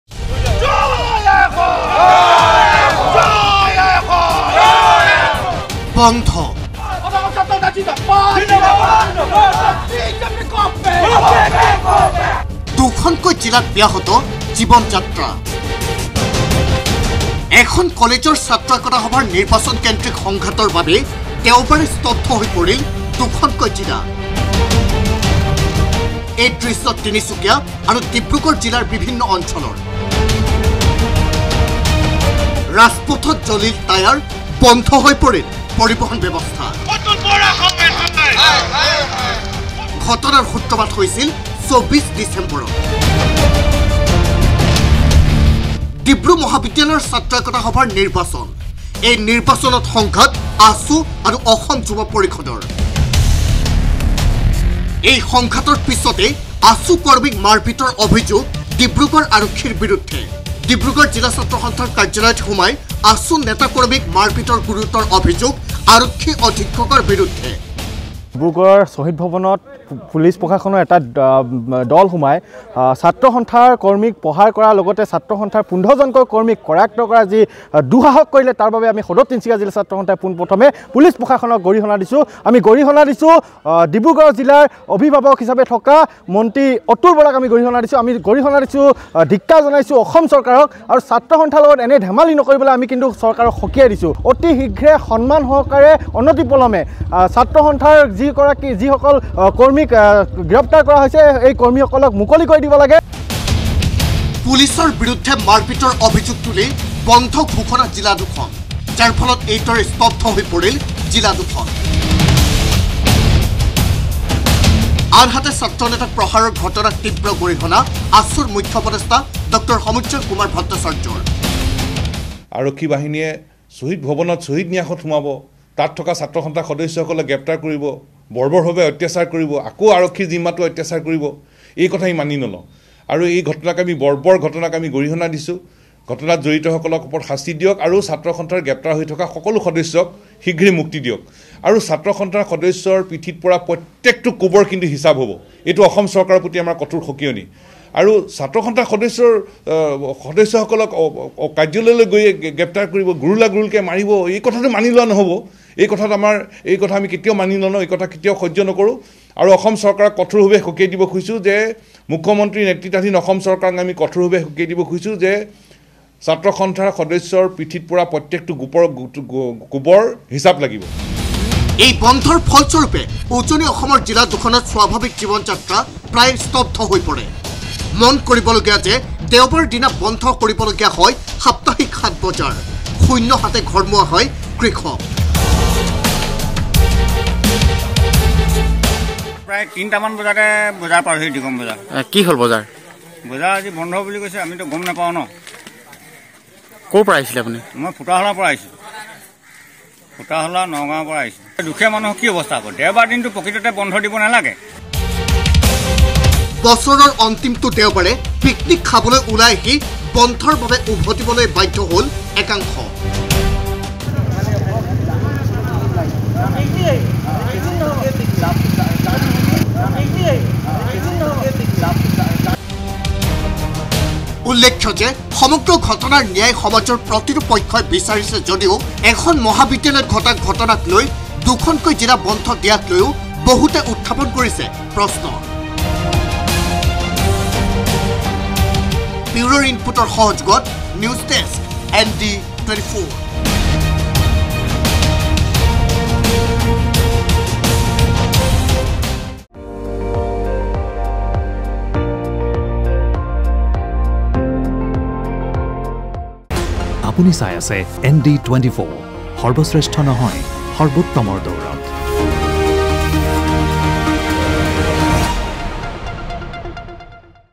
ডিব্ৰু মহাবিদ্যালয়ত নিৰ্বাচনকেন্দ্ৰিক সংঘাত। বন্ধ হৈ পৰিল ডিব্ৰুগড়, তিনিচুকীয়া জিলা
ডিব্ৰু মহাবিদ্যালয়ৰ ছাত্ৰ একতা সভাৰ নিৰ্বাচনক কেন্দ্ৰ কৰি সৃষ্টি হোৱা সংঘাতত বন্ধ হৈ পৰিল উজনি অসমৰ দুখনকৈ জিলা। আছু কাৰ্যালয়তে আছু নেতা-কৰ্মীক ডিব্ৰুগড় আৰক্ষীয়ে প্ৰহাৰৰ অভিযোগত ডিব্ৰুগড় আৰু তিনিচুকীয়া জিলা বন্ধ ঘোষণা কৰিছে সদৌ অসম ছাত্ৰ সন্থাই।